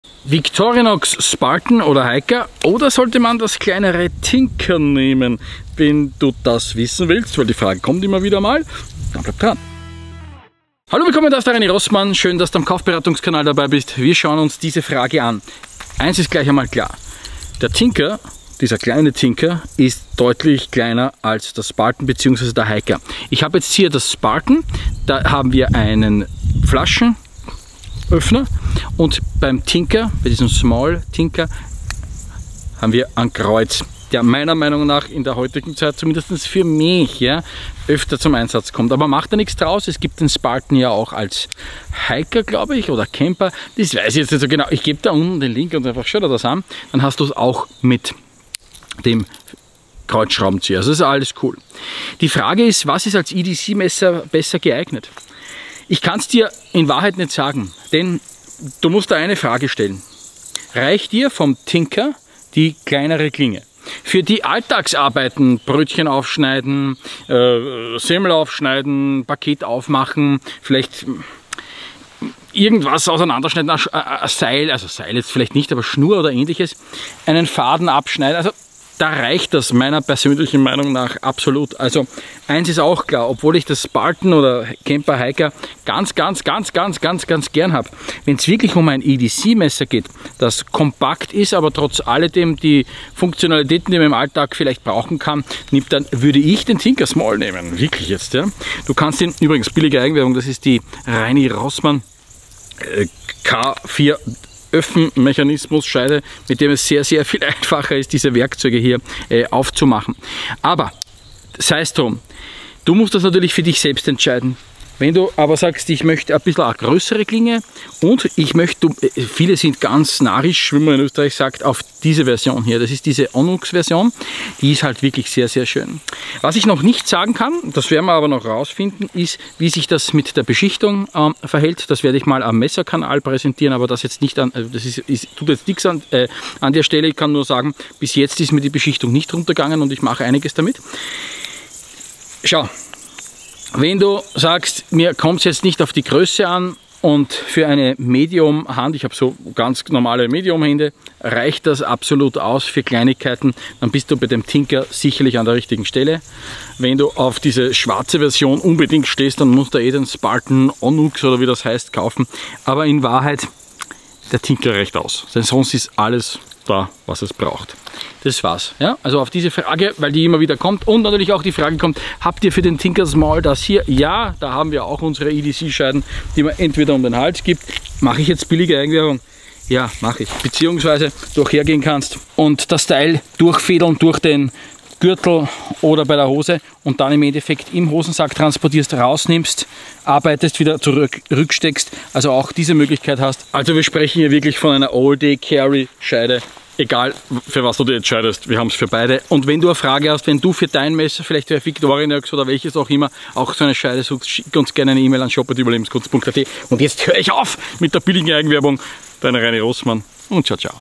Victorinox Tinker oder Spartan/ Hiker / Camper | Reini Rossmann Kaufberatung
Victorinox Tinker
Victorinox Hiker
Wald und Urlaub?